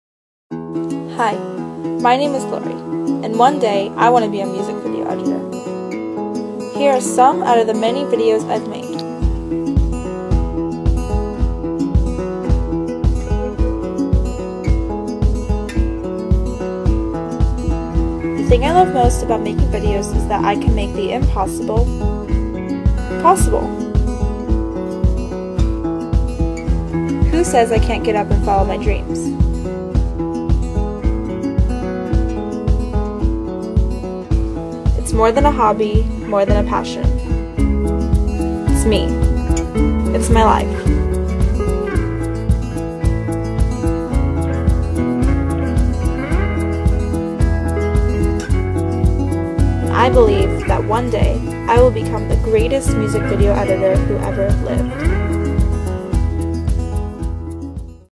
One Day
Acuvue contest :) My dream is to become a Music video editor! I really have a burning passion for this and can't see myself doing anything else! I hope you choose me :) Thank you for giving me this opportunity to share my dream with you! One day..!